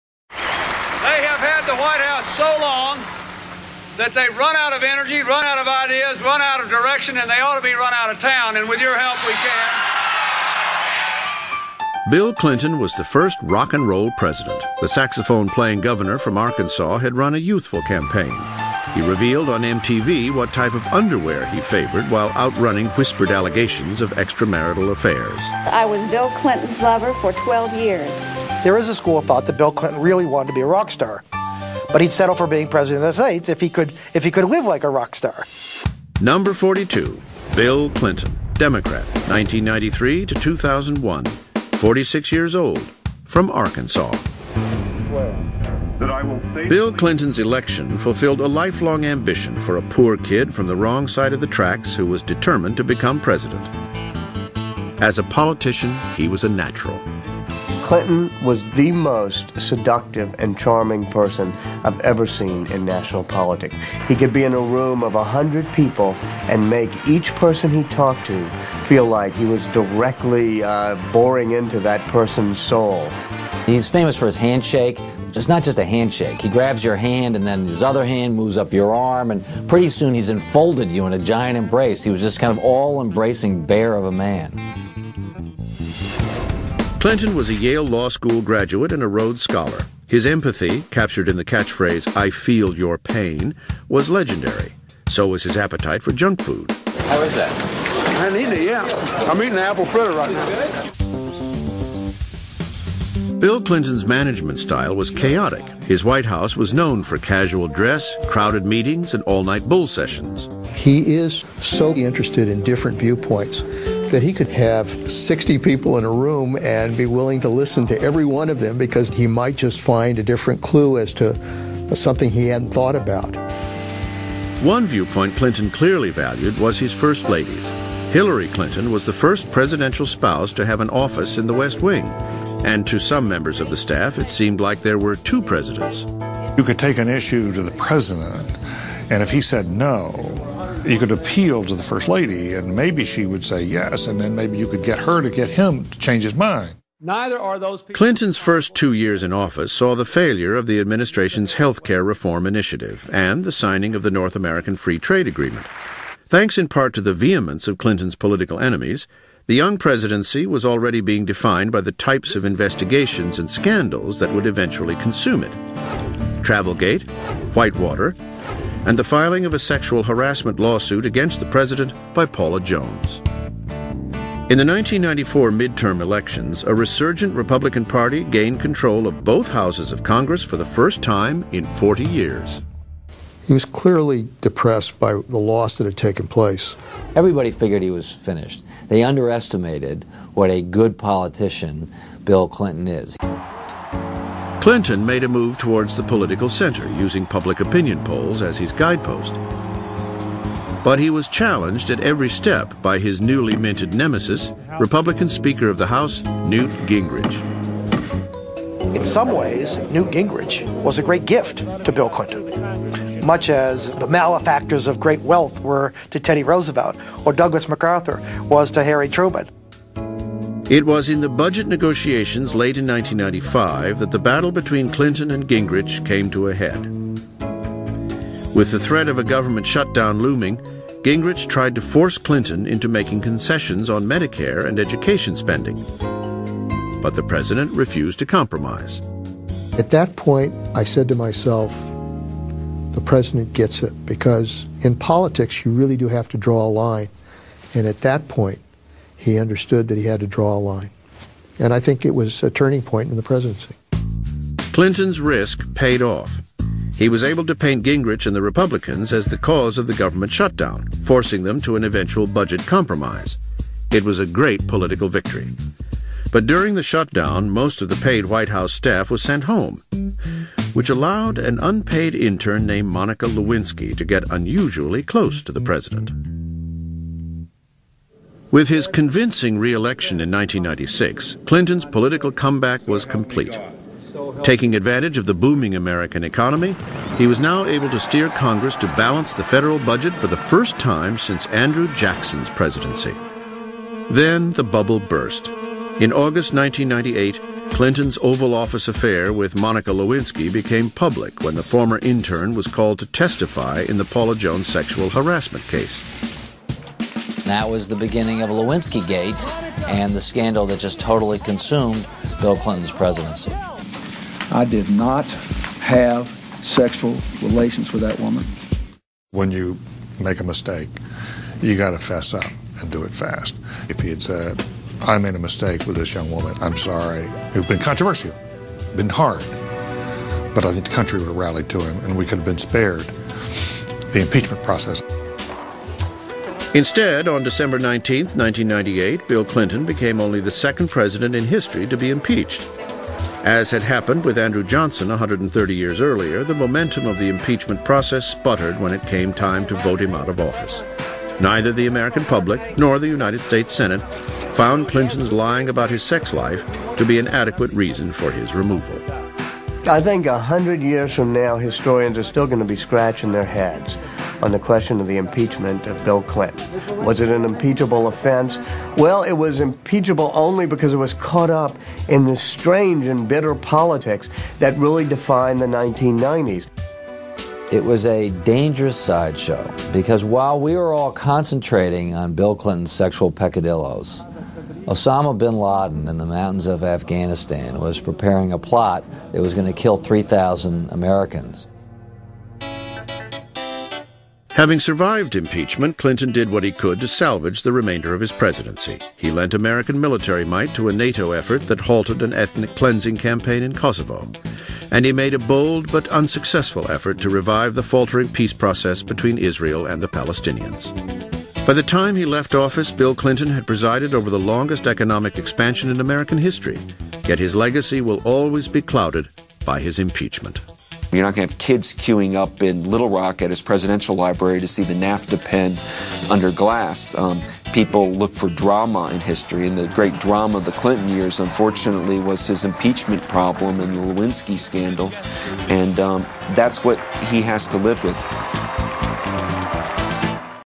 42. W. Clinton - MiniBio_8
This video series gives brief biographical information of U.S. Presidents before / during / after their presidencies.